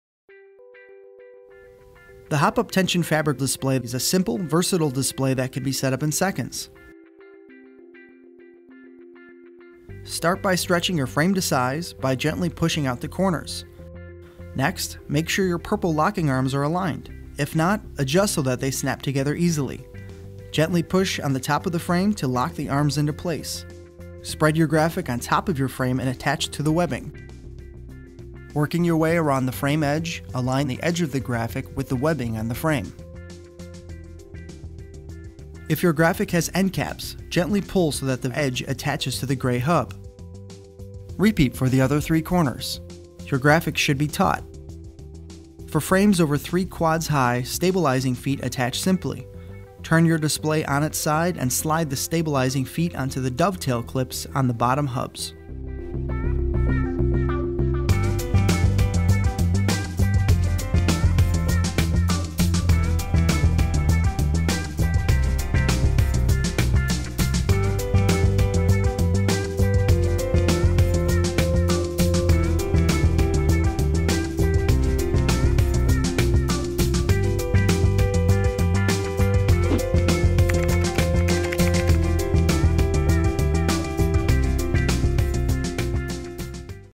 Hopup™ Tension Fabric Graphic Attachment Installation
Hopup™ Tension Fabric displays feature a lightweight collapsible frame and pre-attached tension fabric graphics. This video illustrates how to reattach the fabric graphic to your Hopup frame. The Hopup graphic banner attaches via hook and loop strips, and creates a perfect portable display for use in tradeshows, schools, universities, public buildings, airports, museums, retail environments, corporate offices and more.